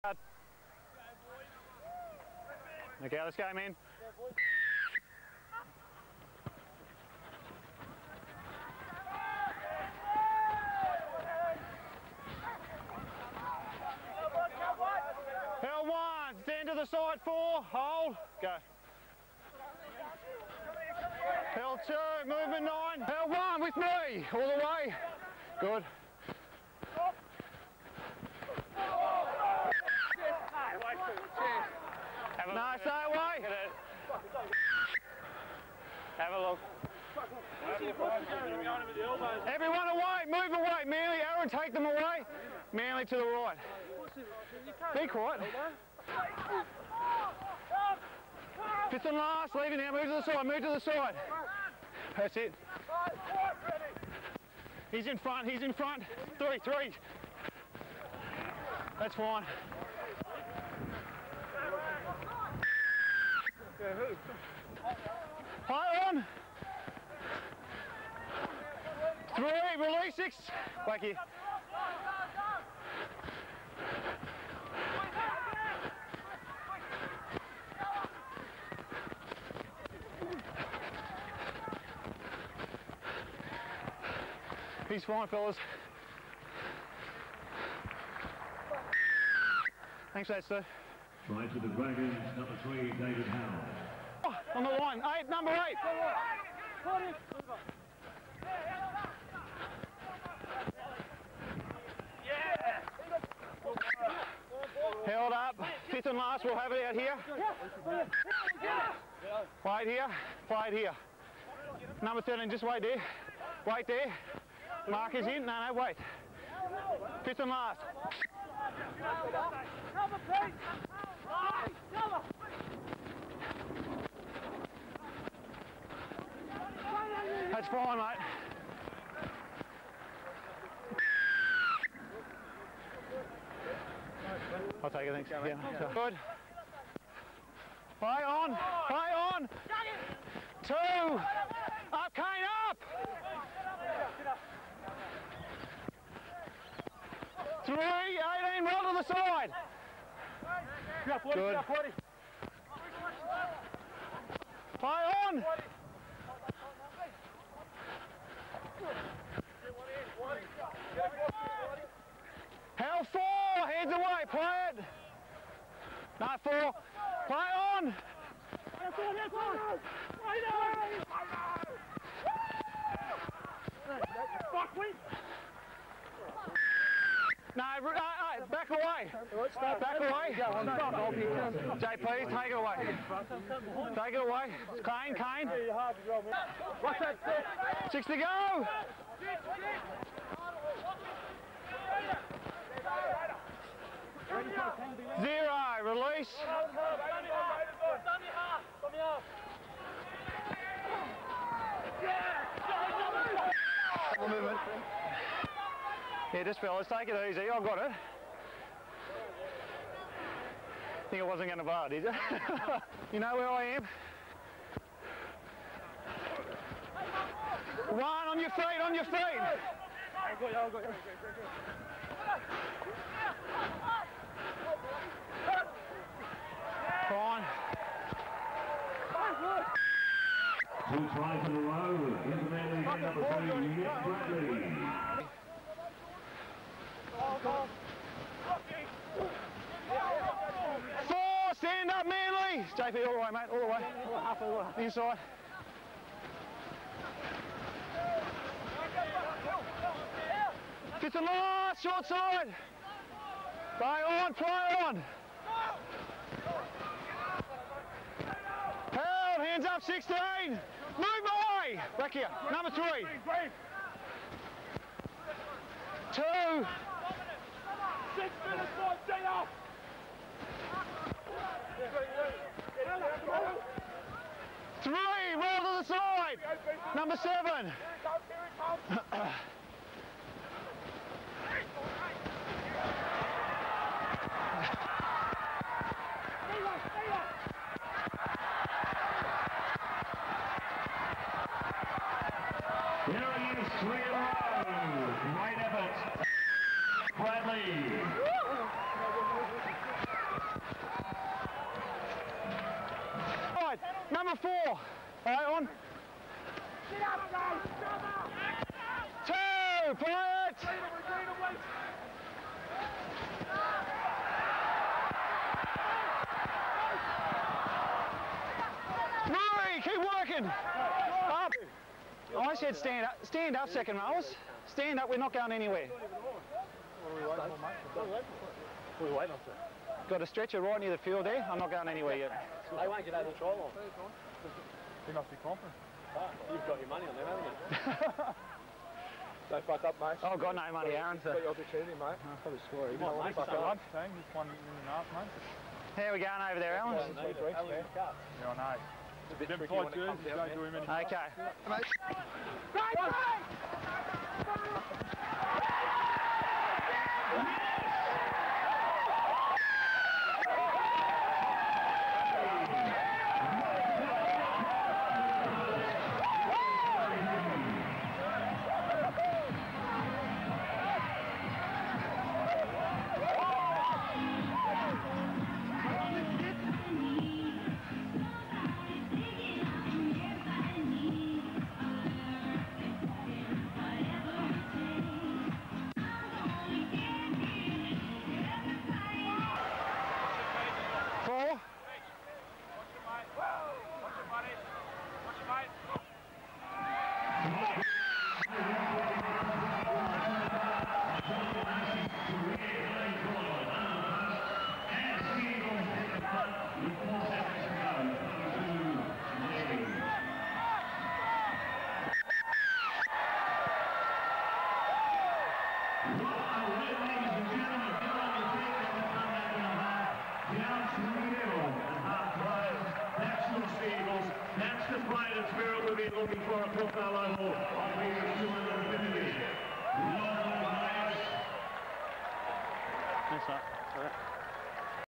20040509 Manly v St George R9 Premier League
Interesting to watch. Manly running 2nd on the ladder on 16 pts and St George 4th on 15 pts. Manly went into the match without starting hooker Hollingsworth who was a late call up to the NRL team.
The ref audio makes the game and you wonder sometimes how they keep up.
St George up 0-20 at half time and things not working well for Manly. Kane Cleal brought into the match to replace Hollingsworth and was constantly chatted to by the ref. Listen to Gorrell (St George) accusing the ref of having money on Manly at the 5:30 mark.
The crowd build up during the 2nd half was noticeable and I am sure helped the Manly players.
Some noteable players running around, DCE's uncle, JP Cherry, Kane Cleal, Paul Stephenson and forum favourite of the time, Phil Morwood and one of my favourites, Mitch Creary, and for St George, Andrew Frew and Aaron Gorrell.